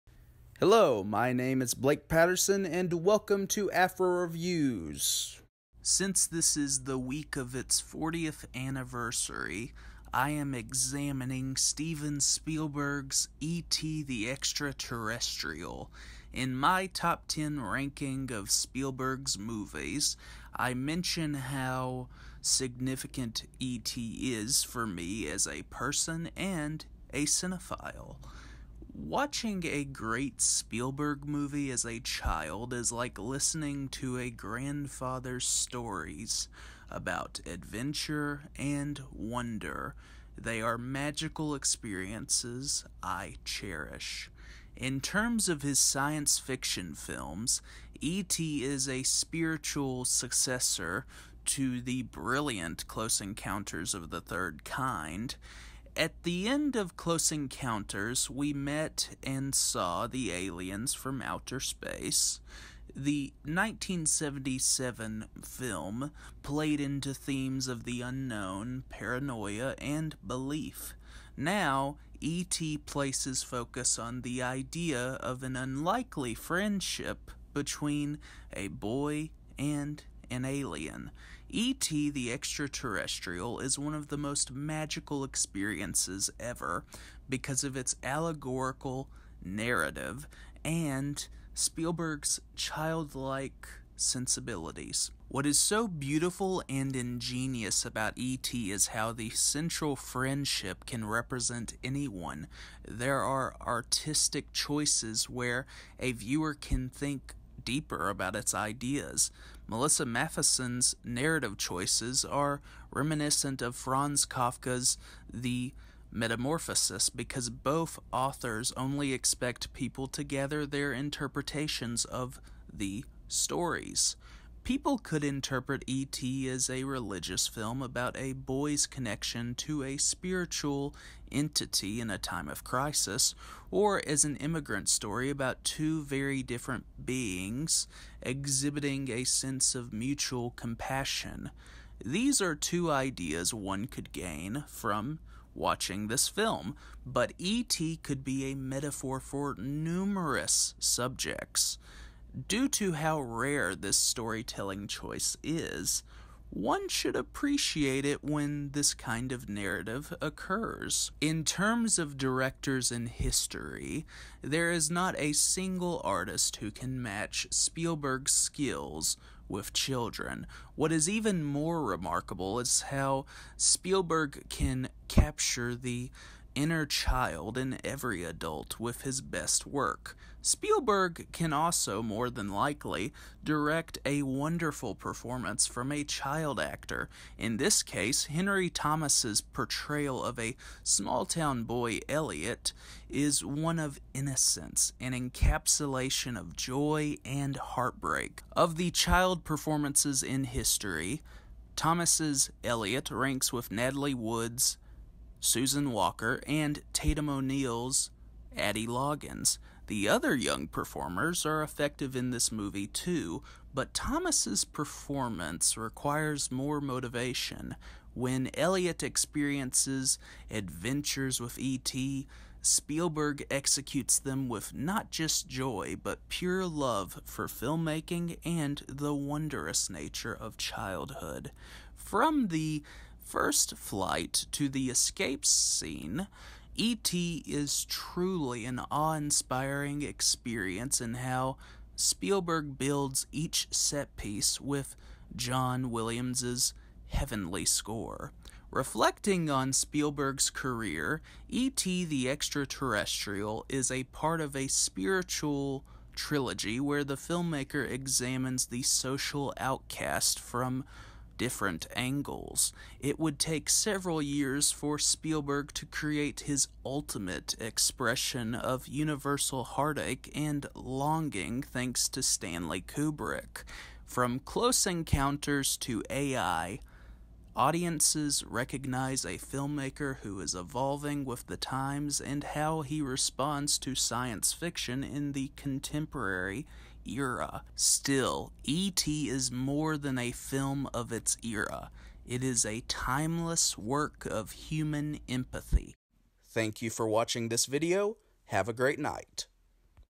Classic Series: E.T. The Extra Terrestrial.
On the latest episode of the classic series, I examine Steven Spielberg’s E.T. The Extra Terrestrial.

I also compose written essays or segments about contemporary films and classics on Letterboxd.